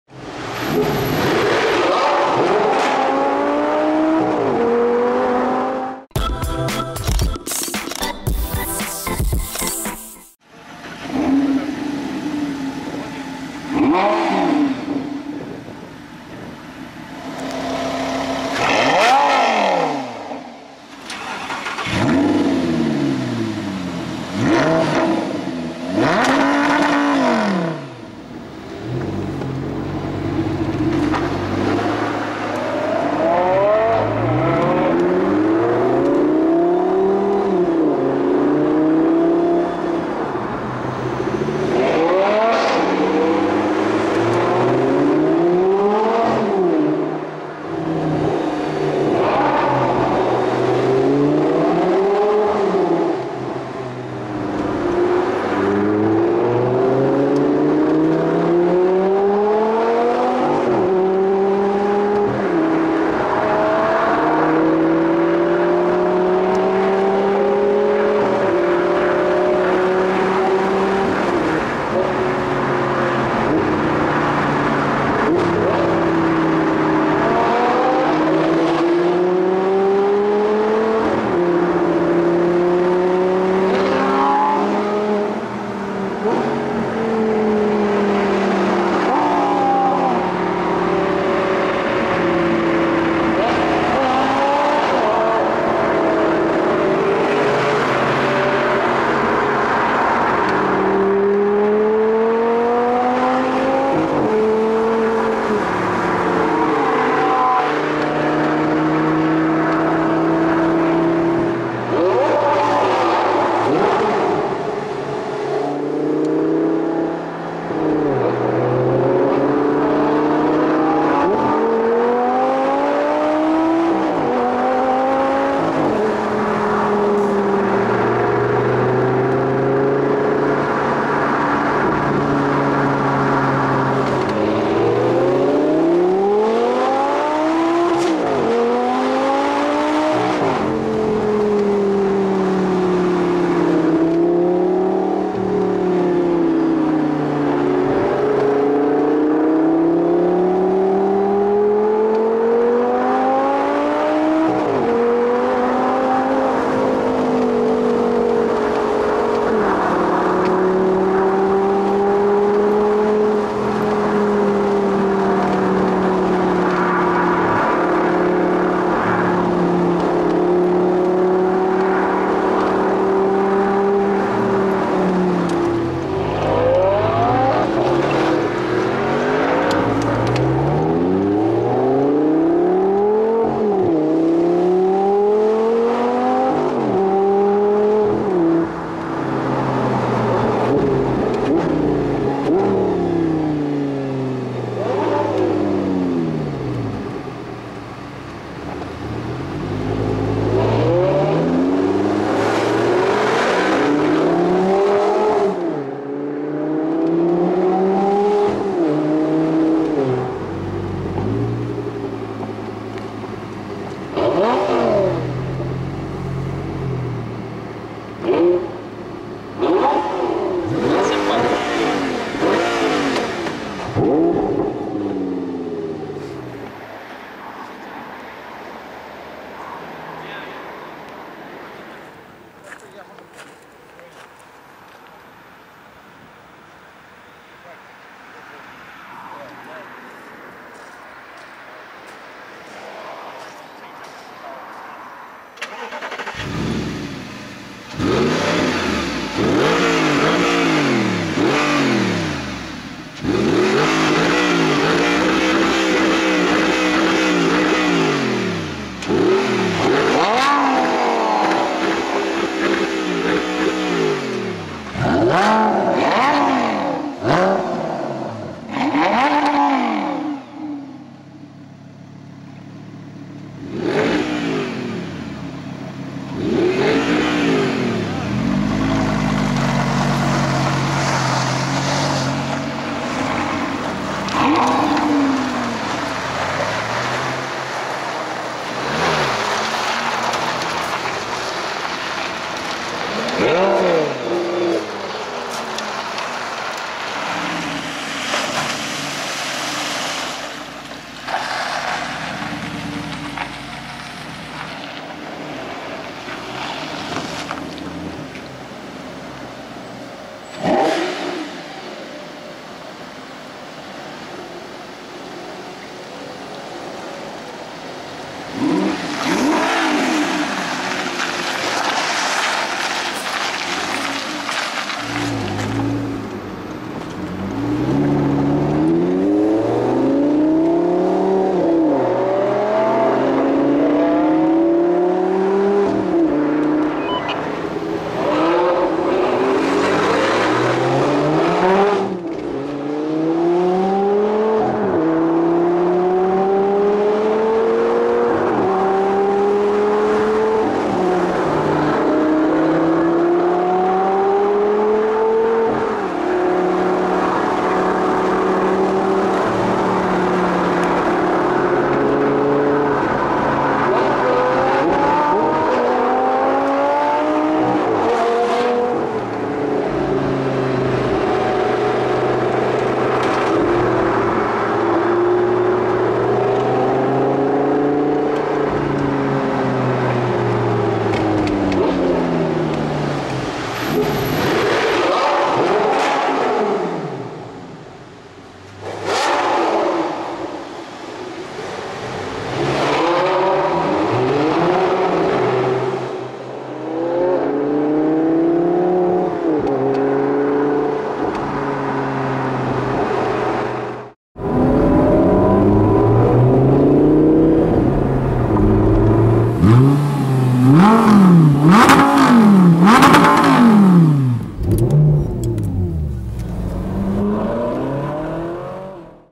Supercar Drive: Performante, QuickSilver R8 V10, Lumo Green R8 V8
I met up with some friends for a little blast out of London. We consisted of my R8 V10 Spyder with my QuickSilver exhaust and KIK Customs midnight purple wrap, a friend who has just had his R8 V8 wrapped by KIK in matte lumo green, and another in the Lamborghini LP570 Performante from Season Car Hire

Enjoy some clips of the cars, flybys, revs, startus, rev-wars, and all sorts!